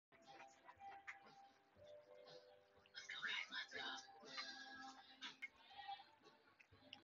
Look what I’m having for lunch
I’m having Alfredo pasta with hot dogs